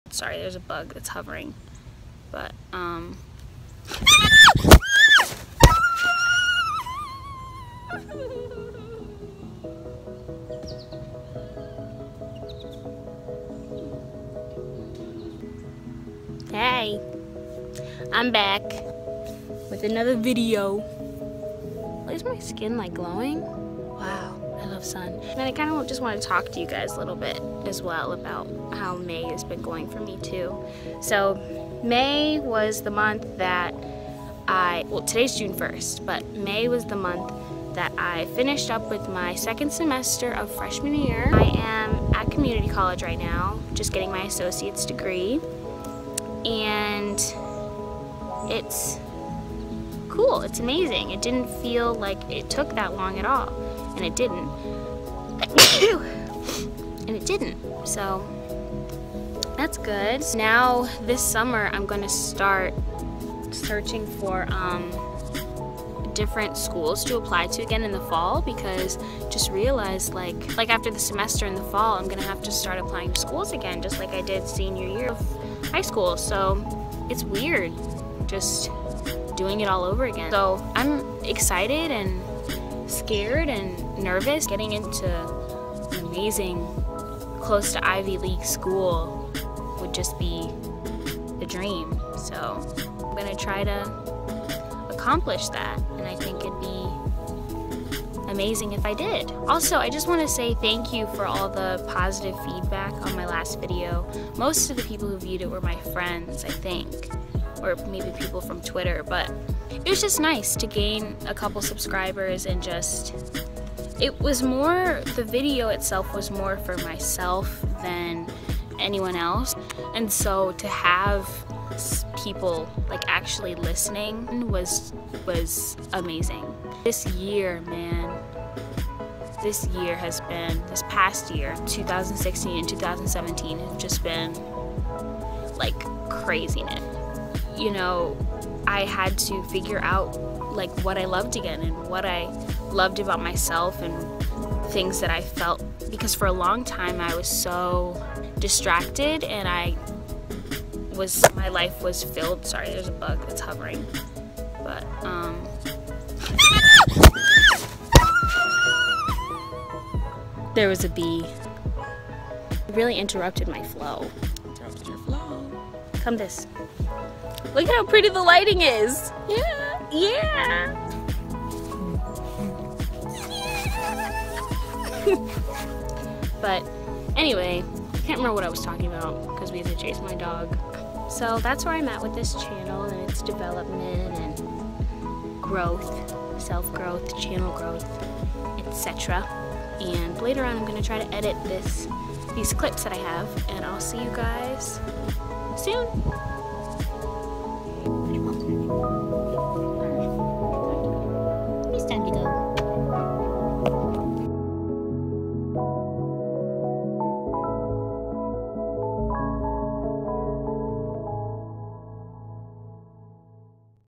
JUNE VLOG | New Channel, College, & Killer Bees.
Hi friends! I'm back with a chill summer vlog. Talking freely and openly about my thoughts on this channel's growth so far and my growth as a person this past year. Hope you all enjoy!

SC: urbffabby